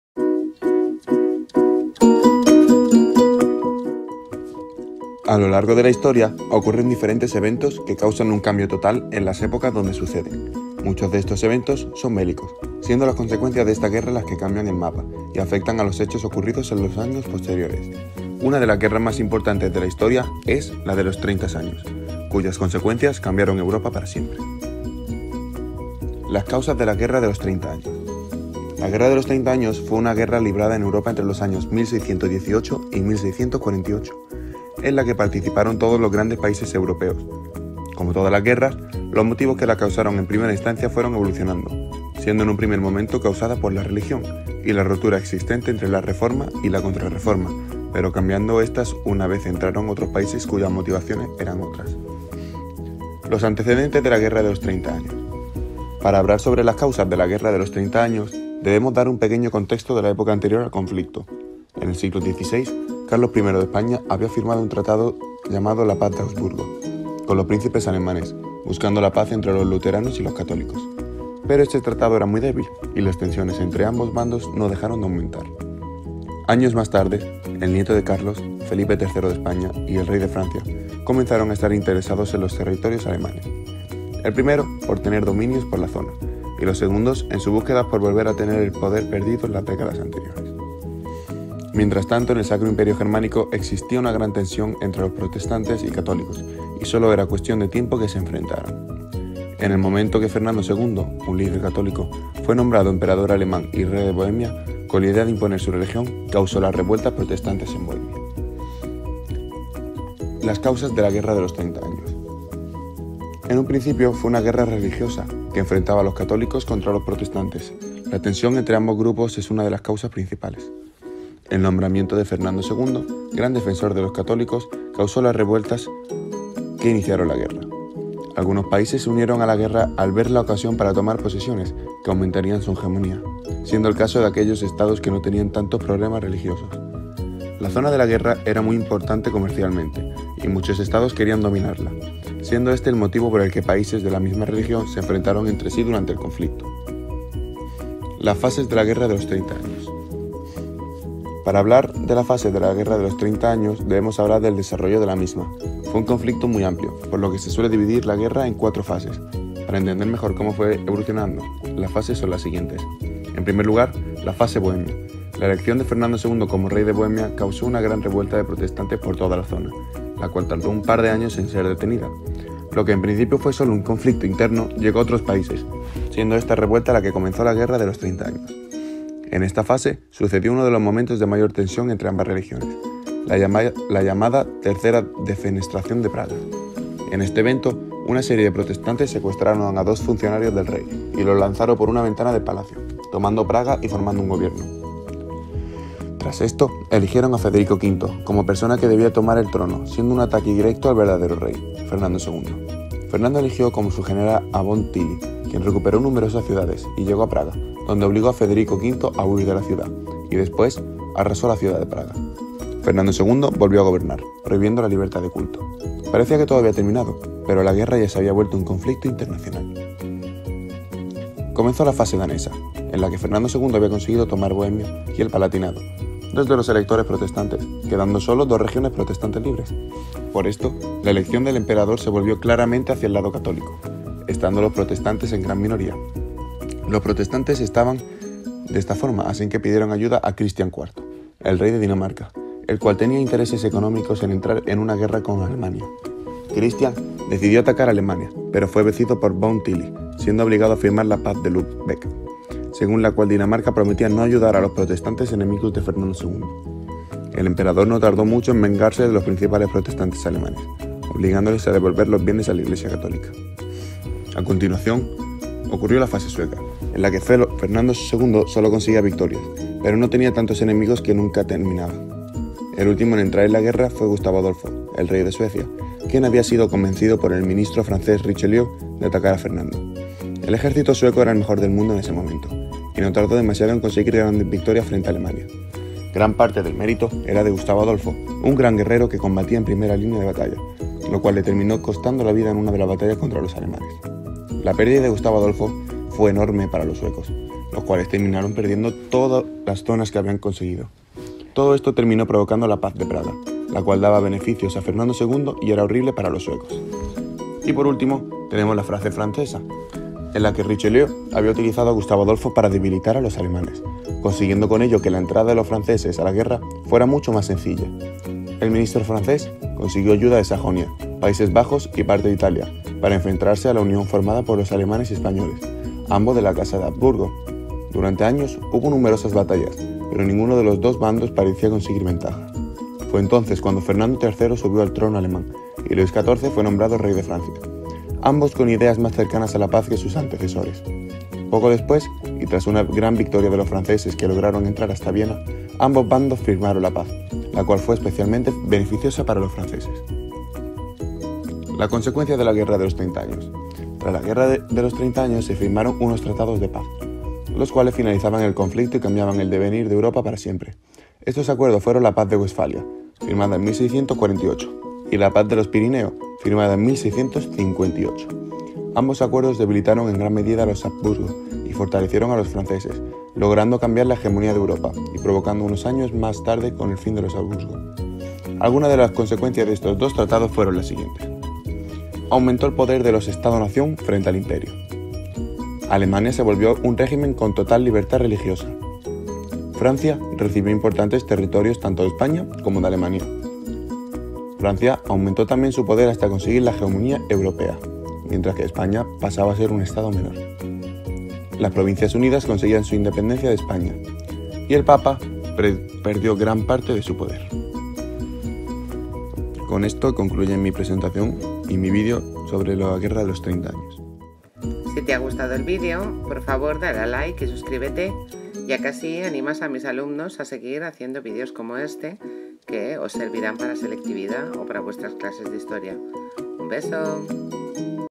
La guerra de los 30 años
Resumen de las etapas de la guerra de los 30 años y La Paz de Westfalia y Pirineos